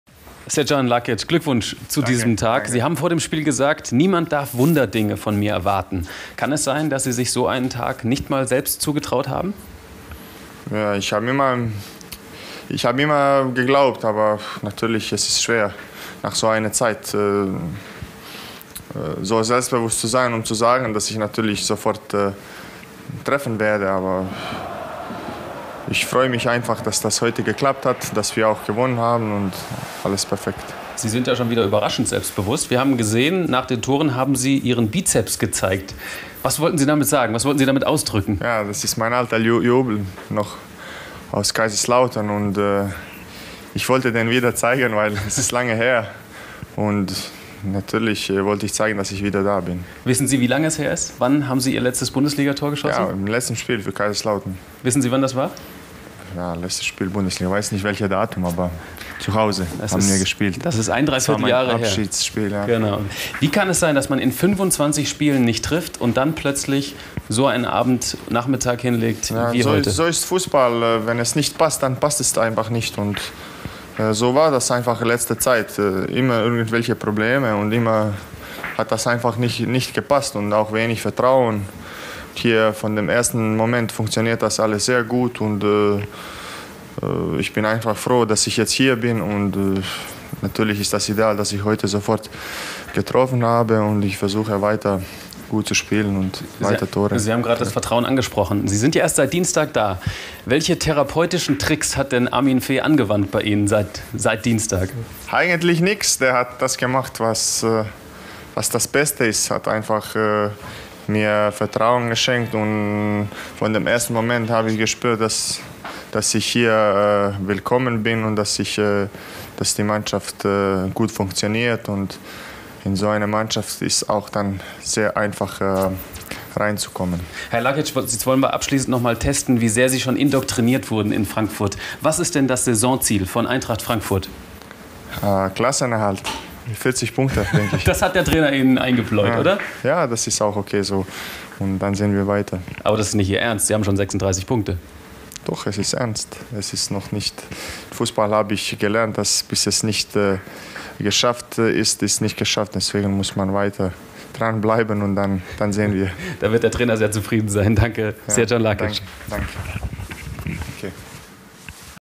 Srdjan Lakic: "Bin wieder da"
Nach seinen beiden Toren gegen den HSV erklärt Srdjan Lakic, wie man als Stürmer nach 25 torlosen Spielen wieder so treffen kann. "Der Trainer hat mir Vertrauen geschenkt", sagt er im ZDF-Interview.